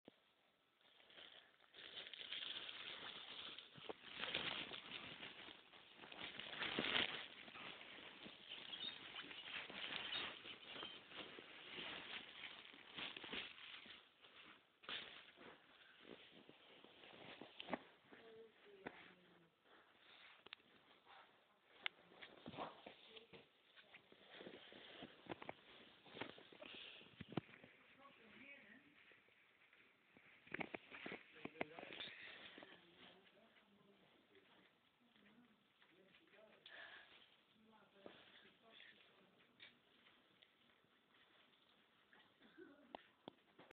Me Opening My New High School Musical Cushion.
Just a little video of me opening my new High School Musical cushion!